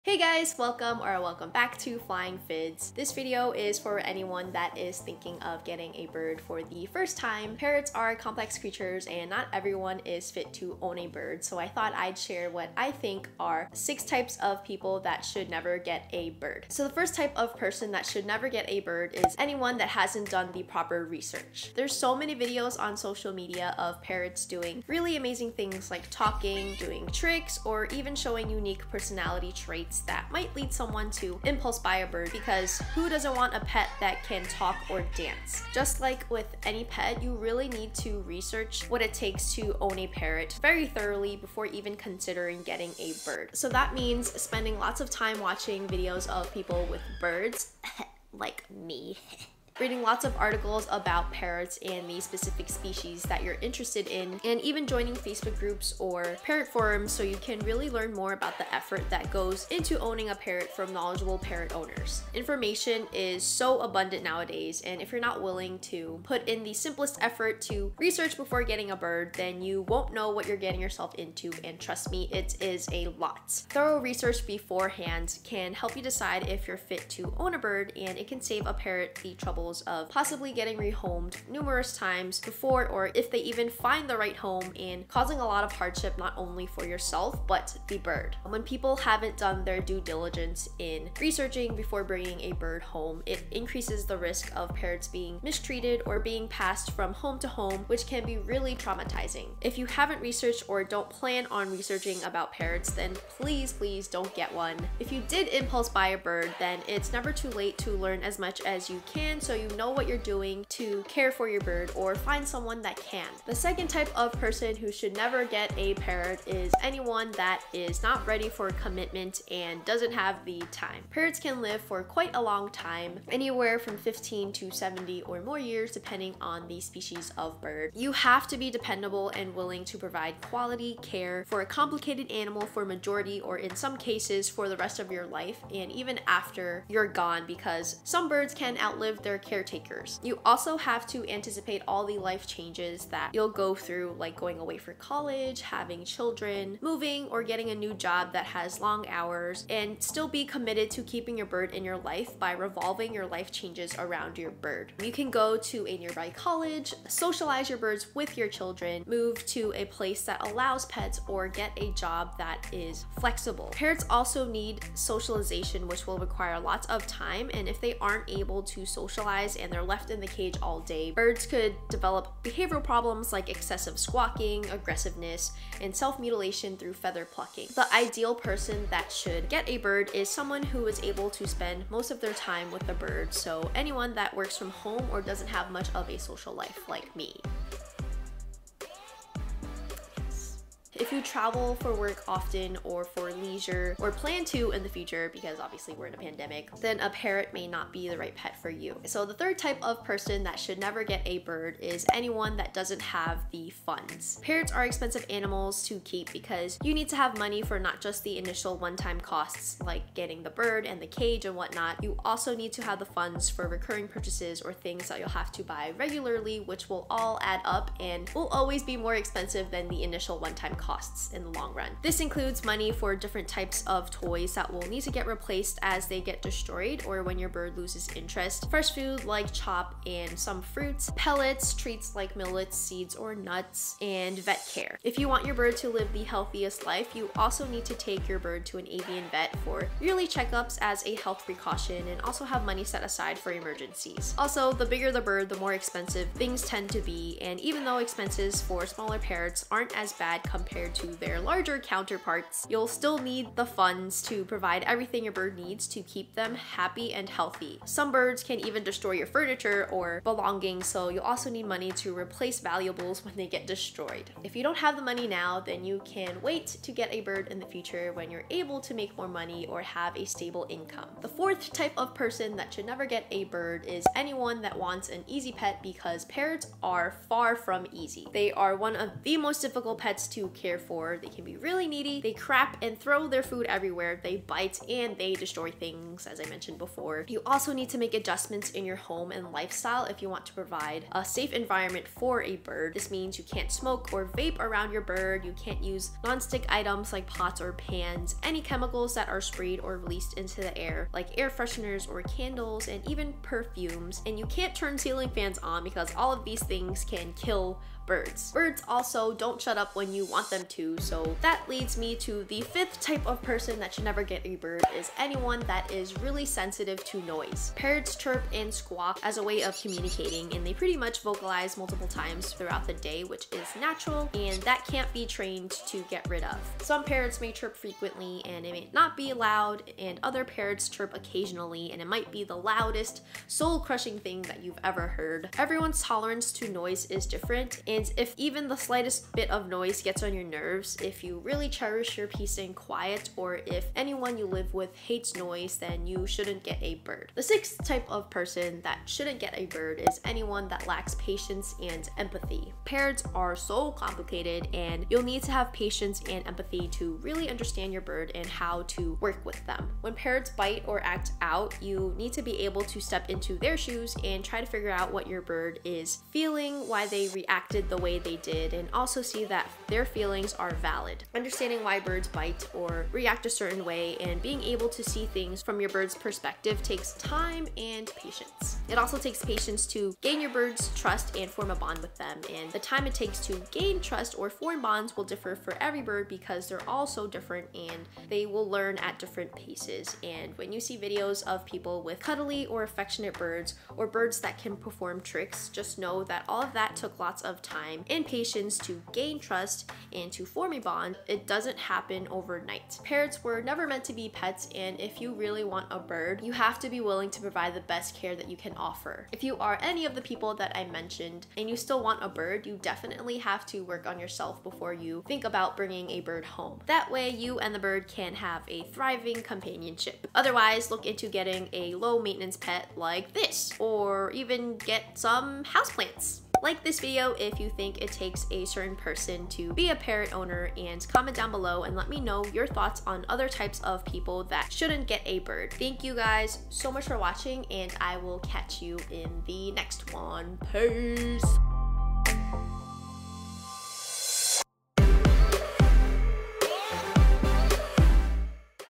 SHOULD YOU GET A BIRD? 6 TYPES OF PEOPLE THAT SHOULD NEVER GET A BIRD
Parrots are complex creatures and not everyone is fit to own a bird so I wanted to share my thoughts on 6 types of people that should never get a bird. Parrots were never meant to be pets and unless you are willing to commit to them for many years to come and provide the best care, then please don't get a bird!

🐥My little chickens🐥
💛 Pineapple 💛: pineapple green cheek conure
💙 Pumpkin 💙: turquoise green cheek conure

(I earn from qualifying purchases at no additional cost to you! Any money earned goes to getting Pinny and Pumpkin new toys and food!)

xo
Won, Pineapple, and Pumpkin!